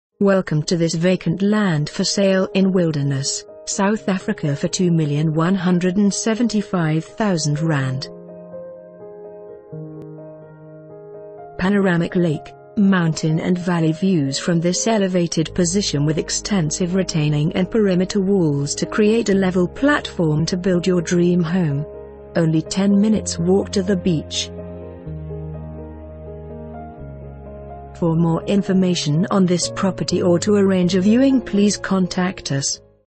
Vacant Land For Sale in Wilderness, South Africa for ZAR 2,175,000...
Panoramic lake, mountain and valley views from this elevated position with extensive retaining and perimeter walls to create a level platform to build your dream home. Only 10 minutes walk to the beach.